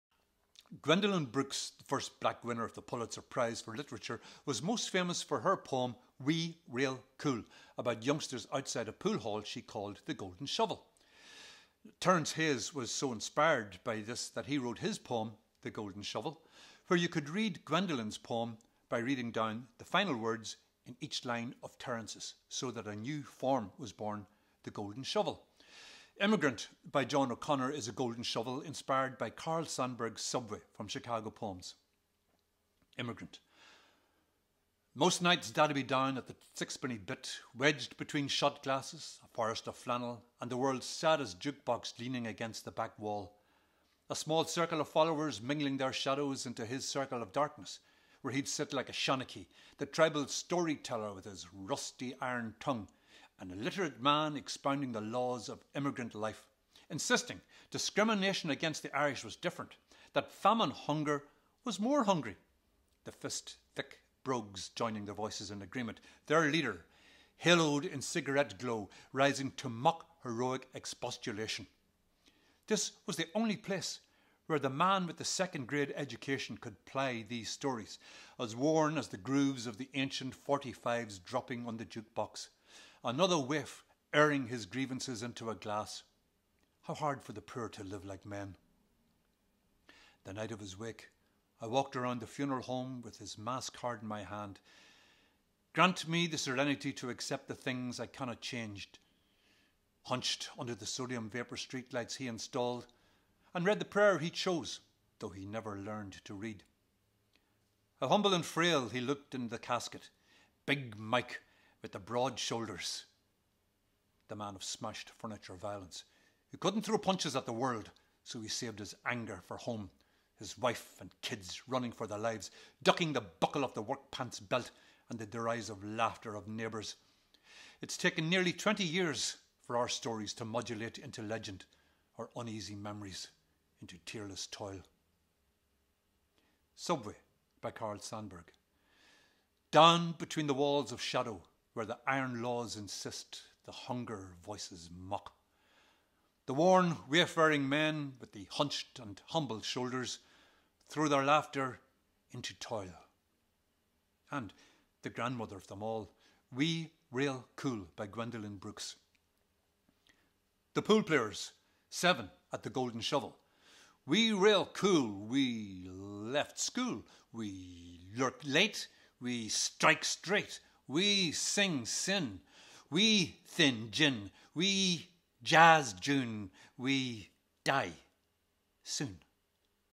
Immigrant by John O'Connor
Gwendoline Brooks begat Terrance Hayes begat Carl Sandburg begat @john_s_oconnor!
The development of The Golden Shovel form and its progeny.
The Golden Shovel explained & Immigrant by John O'Connor offered for Covid19Poetry SouthArmagh @poetryireland